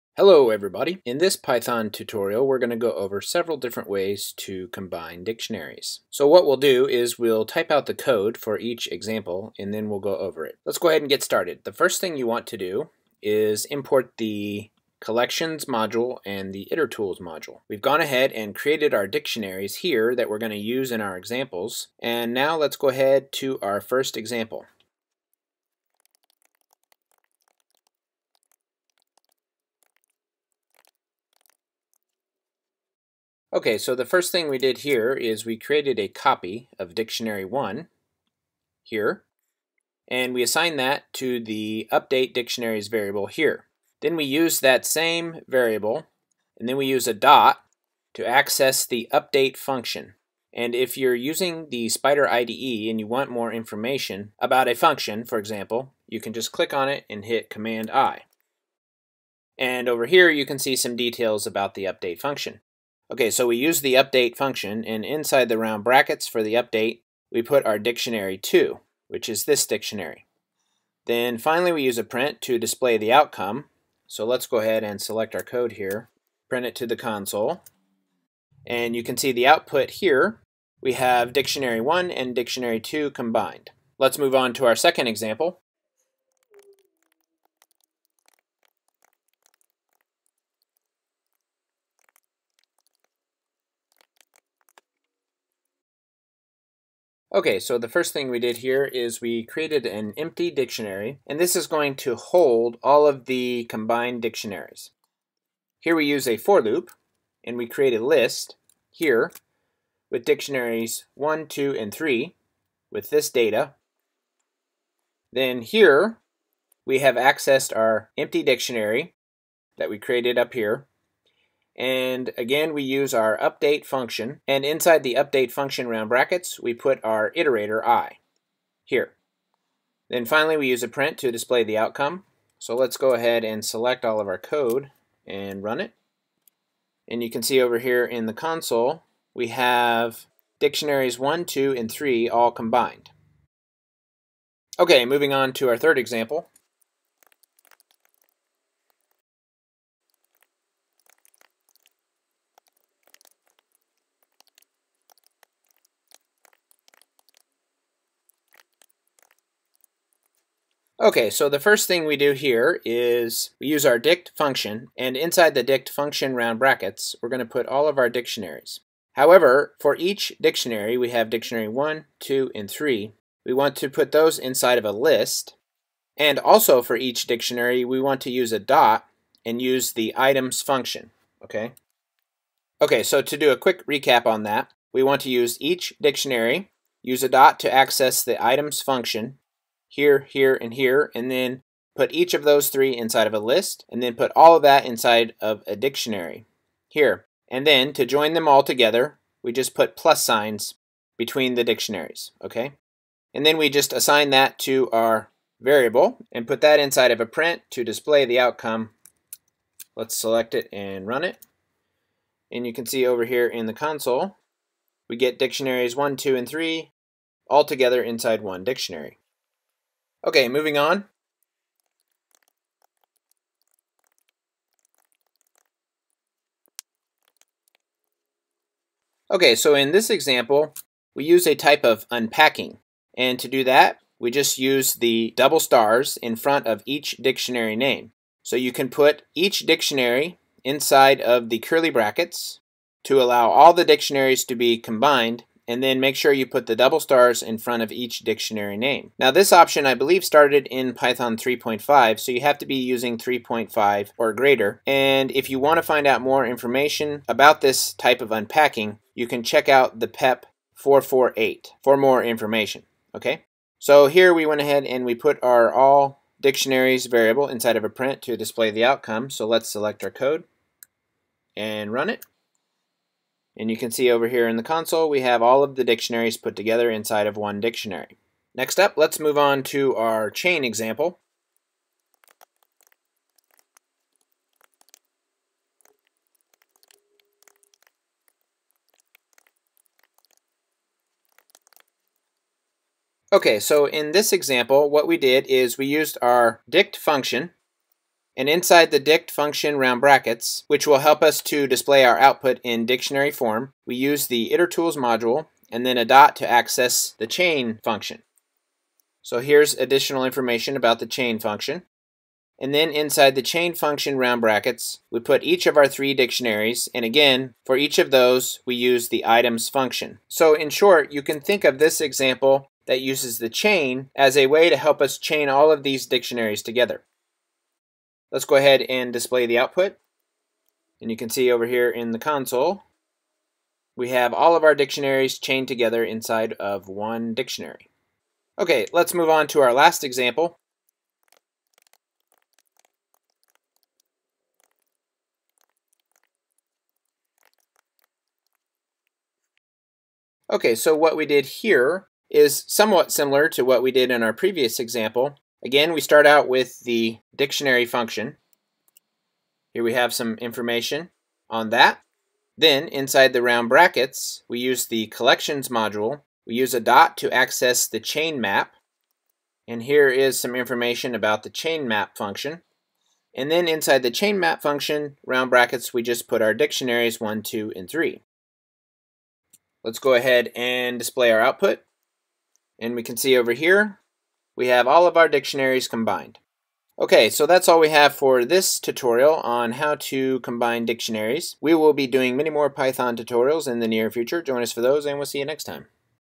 Python Combine Dictionaries
In this Python tutorial, we will go over several ways to combine (add together or concatenate) dictionaries.